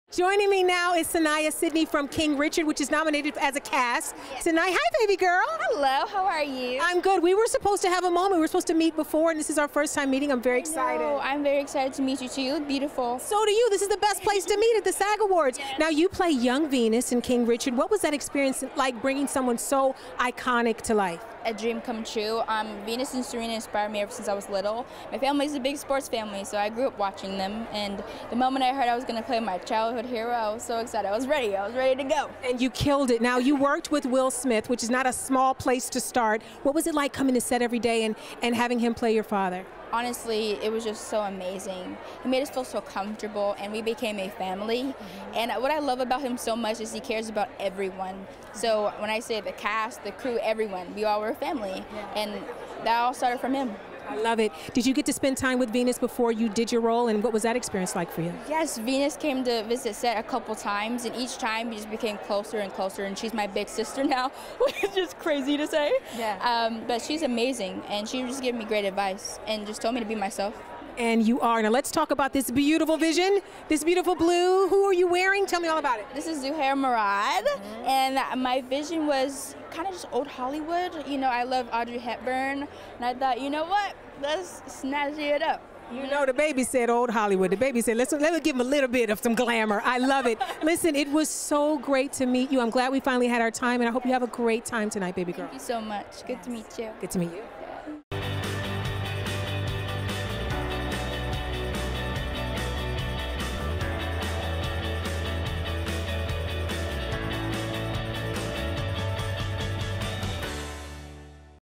Saniyya Sidney Talks Playing Venus Williams in "King Richard" | E! Red Carpet & Award Shows
Saniyya Sidney reveals what it was like to work with Will Smith and shares her excitement playing Venus Williams at the 2022 SAG Awards.